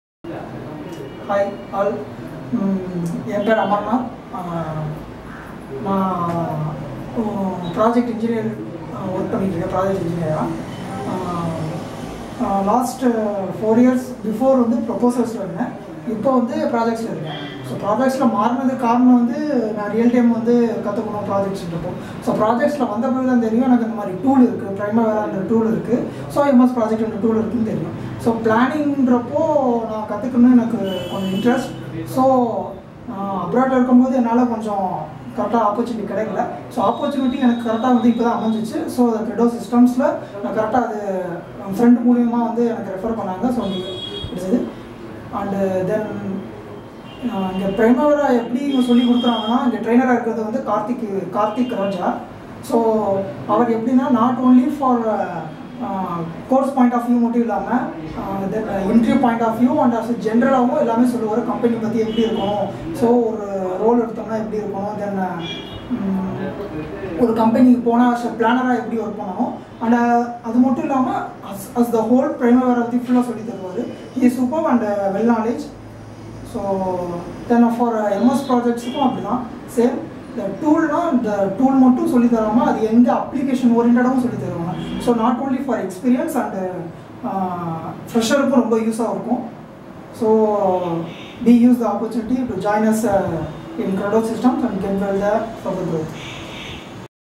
CREDO SYSTEMZ - PRIMAVERA P6 & MS PROJECT TRAINING's - Amarnath - Video Review & Feedback
The above is a video review and feedback given by a candidate who has completed the training with us, CREDO SYSTEMZ.

All the opinions and feedback given in the video are own words of the candidate.